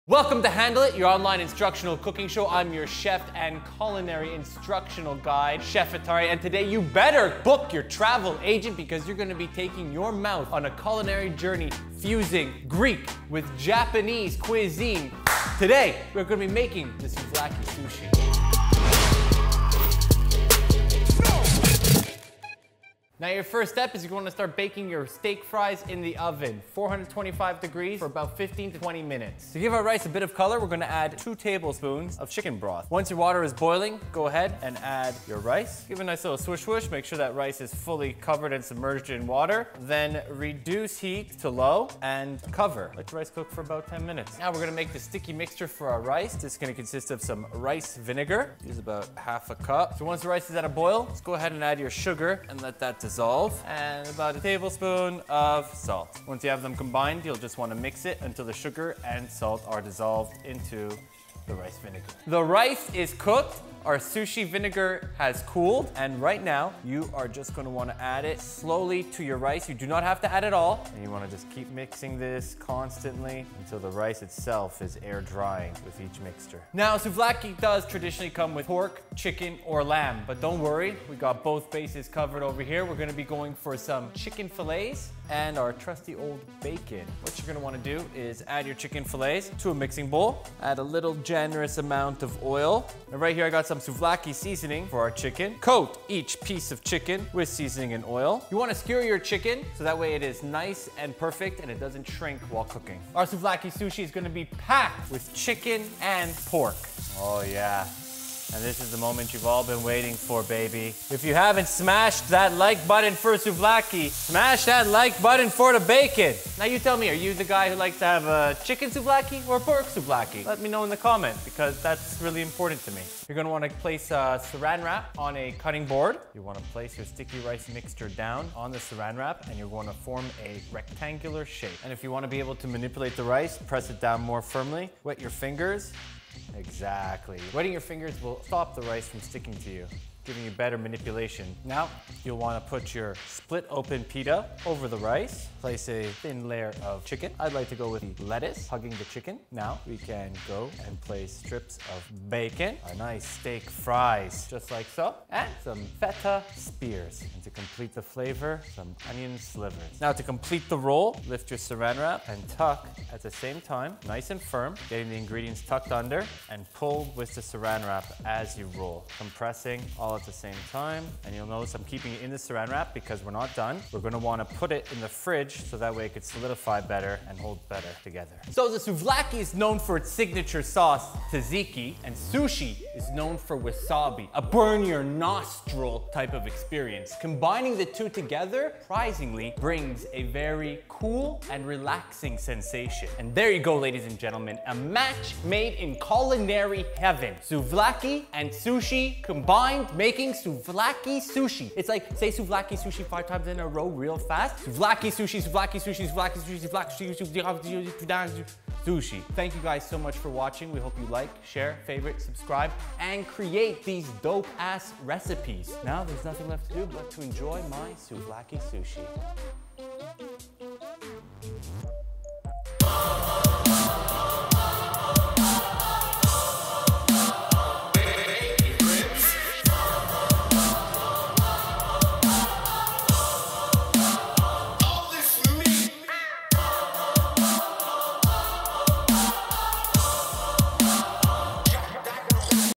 Sushi Souvlaki - Handle it
This week Chef Atari teaches you how to make this unique creation "Sushi Souvlaki"

Ingredients
1/2 Pack of frozen french fries
3 Tbsp of chicken stock
2 Cups of rice
1/2 Cup of rice vinegar
3/4 Cup sugar
1 Tsp of sat
8 Chicken filets
Chicken seasoning
3 Tbsps of Vegetable oil
1/2 Pack of bacon
6 Pitas
Shredded Lettuce
Feta cheese
Slivered Onions
1 Container tzatziki
1 Tsp wasabi

Tools
2 Small sauce pans
2 Frying pans
1 Chef knife
1 Cutting Board
Skewers
Seran wrap
1 Oven tray
Tin foil
1 Whisk
1 Pair of tongs
2 Mixing bowl

Step 1
Bake french fries in the oven at 425 degrees Fahrenheit for 15 minutes until crispy.

Step 2
Boil 2 cups of water in a small sauce pan and season with 3 Tbps of chicken stock. Add 2 cups of rice to boiling water and let cook for 6 to 8 minutes on low heat.

Step 3
Boil 1/2 a cup of rice vinegar in a small sauce pan. Add 3/4 cups of sugar, 1 Tsp of salt and stir well until sugar dissolves. Cool, then stir into the rice until fully coated. Only pour half of rice vinegar on rice add more if needed.

Step 4
Season chicken and skewer then cook in a frying on medium heat for 8 minutes until cooked.

Step 5
Cook bacon strips on a frying pan for 10 minutes until crispy.

Step 6
Lay down sticky rice on a cutting board wrapped in seran wrap. Split in half 2 pitas and place on top of rice. Add chicken, lettuce, bacon, feta, french fries and onion. Fold pita over and begin to roll with seran wrap theb Refrigerate for 30 minutes.

Step 7
Mix 2 Tbps of tzatziki with 1 Tsp of wasabi and serve with sushi.

LIKE/FAVORITE and SHARE for new meals every week!